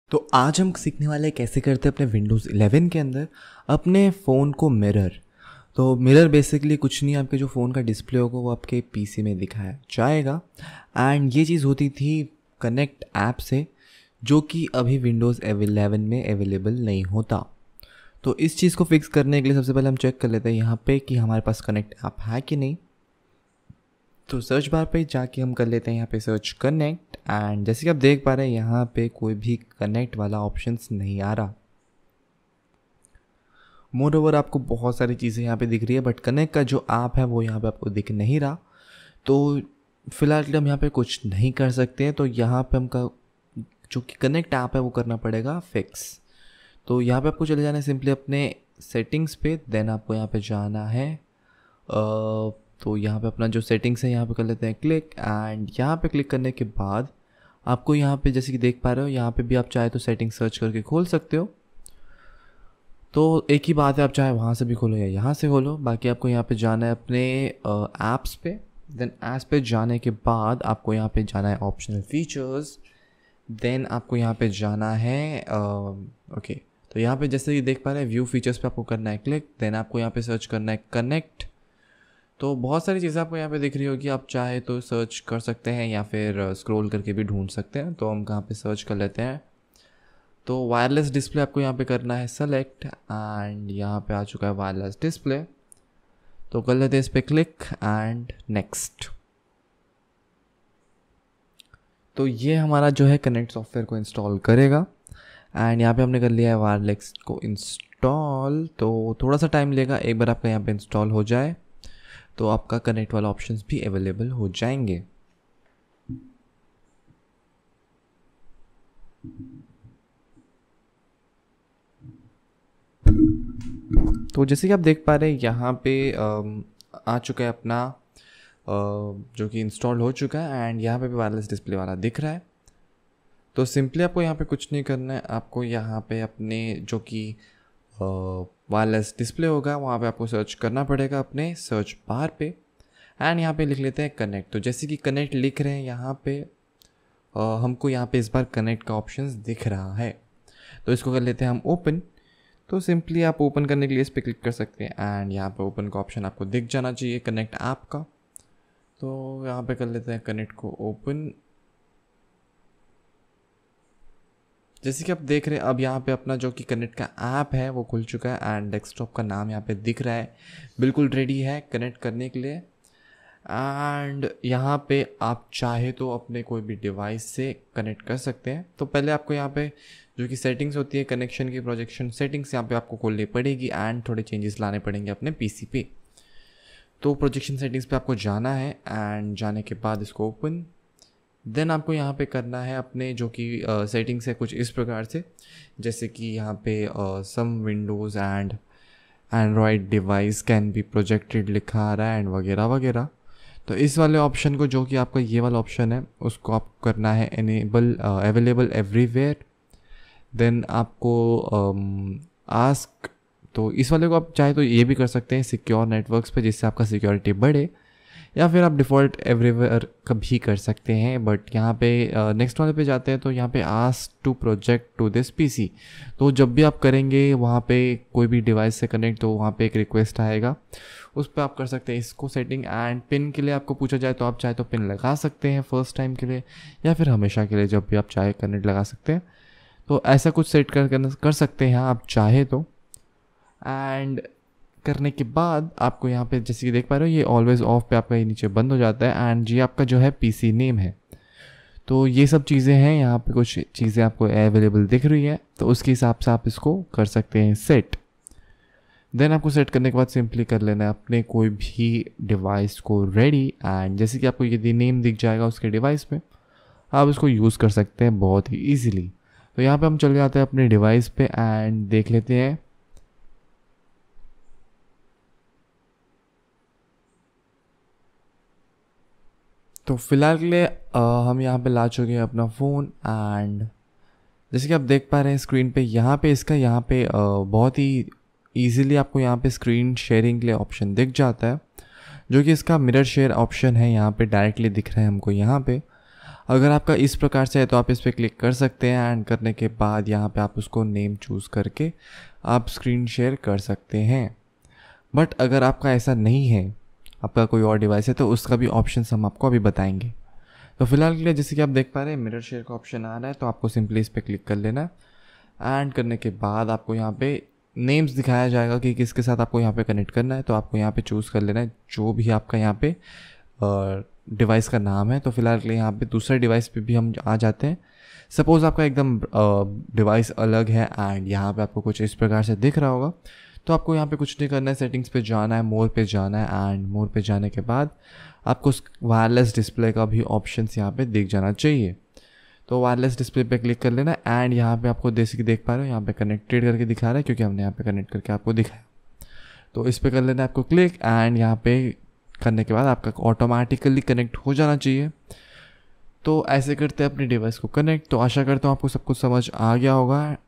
How to Mirror Phone on Windows 11 PC in hindi
in this video we are going to learn about How to Mirror Phone on Windows 11 PC in hindi